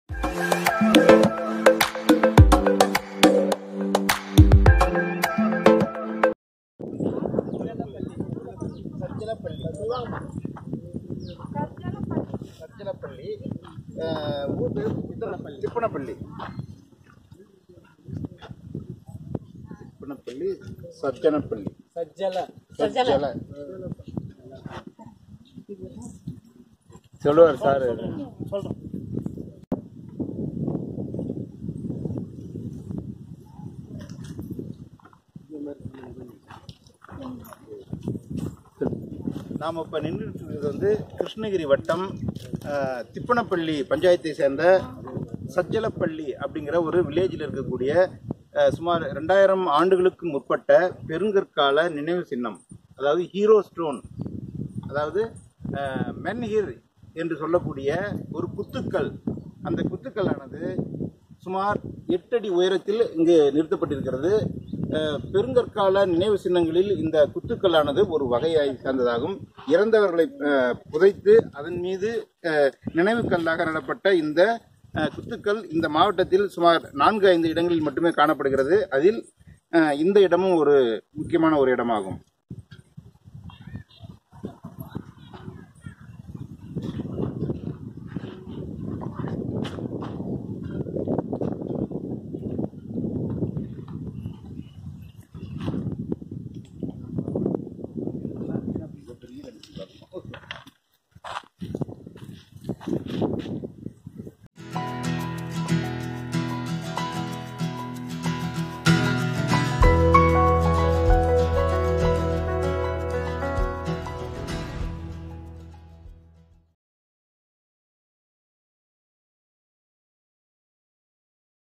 120-2500 years old MENHIR - தாசரிப்பள்ளி அருகே -குத்துக்கல் KHRDT
தகவல் கொடுத்த திருவேங்கடம் அவர்களுக்கு நன்றி.தாசரிப்பள்ளி செல்லும் வழியில் சஜ்சலப்பள்ளி அருகே உள்ள மாரியம்மன் கோவில் அருகே 2500 ஆண்டுக்ளுக்கு முற்பட்ட குத்துக்கல் இன்று வரலாற்று ஆய்வு மற்றும் ஆவணப்படுத்தும் குழுவால் கண்டறியப்பட்டது. அது பற்றி மக்களுக்கும் கூறினோம்.குத்துக்கல் (menhir) அல்லது நெடுங்கல் என்பது பெரிய நிலைக்குத்தாக நாட்டப்படுகின்ற தனிக் கல்லாகும். இது வரலாற்றுக்கு முற்பட்ட காலம் முதலாகவே இறந்தோருக்காக அமைக்கப்படும் நினைவுச் சின்னமாகும்.